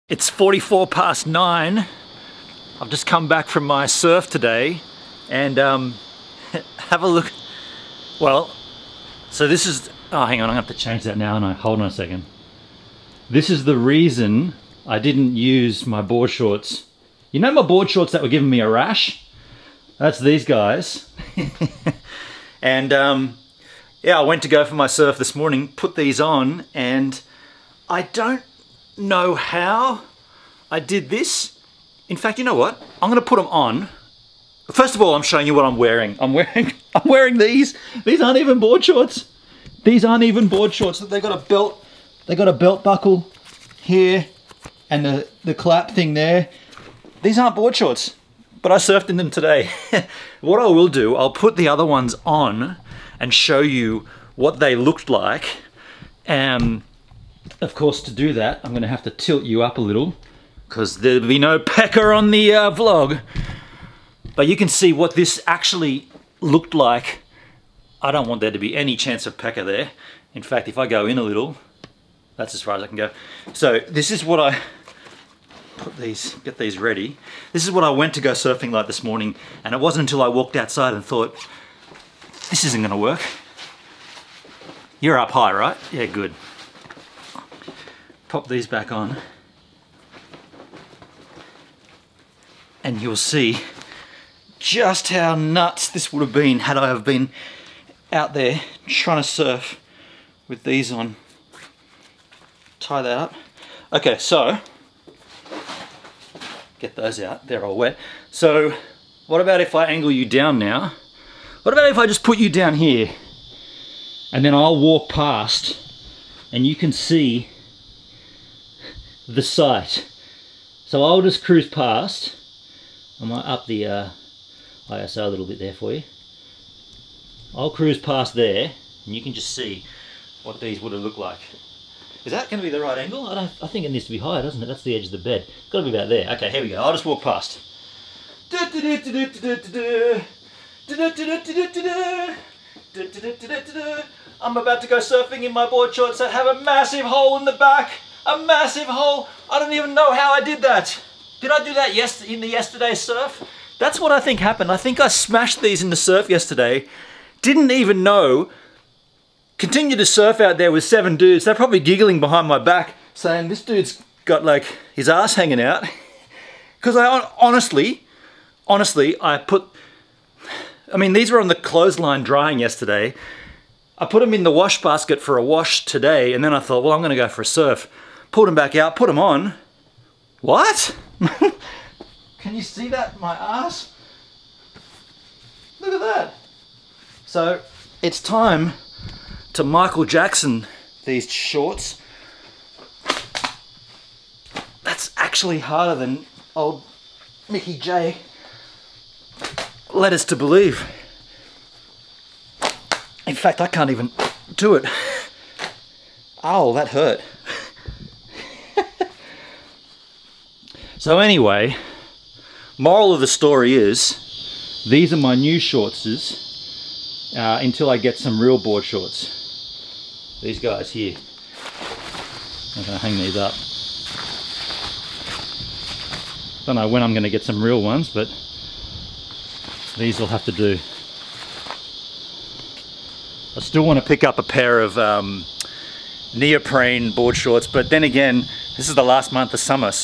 It Might Be Time For New Board Shorts
I have been holding out, but it might now be time for new board shorts! If you're a photographer you may find my Lightroom Presets handy

This is daily vlog 867 since I began this channel in August 2015 and today between 09:44 and 09:48 I was home in Culburra Beach finally deciding that maybe it might be time for new board shorts.